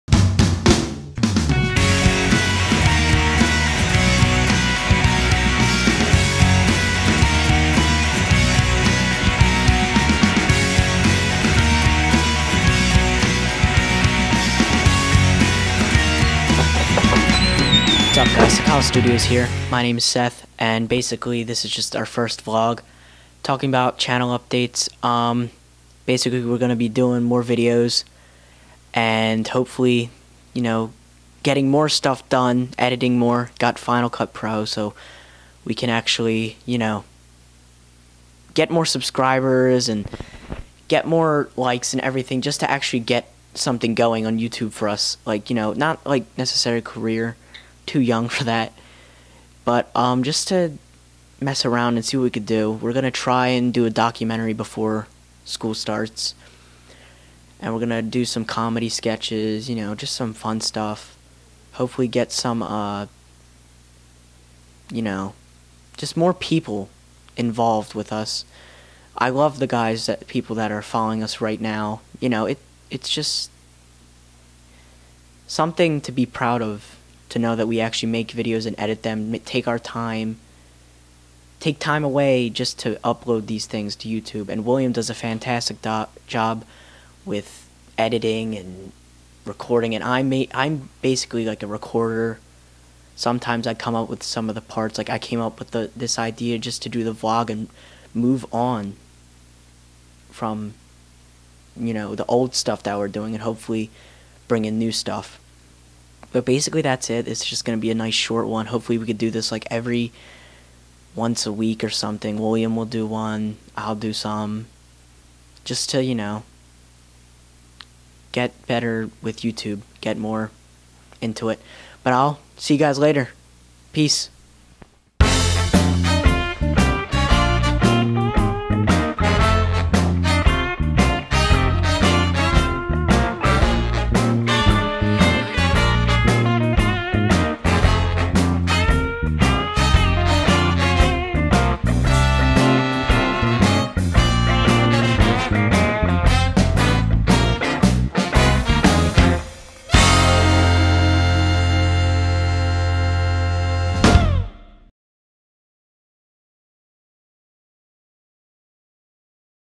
Update: We're doing an Update Vlog
Some info about the channel... So it's an update as the name implies. In this video Seth highlights some changes we are planning to undergo as well as the ones that have already happened!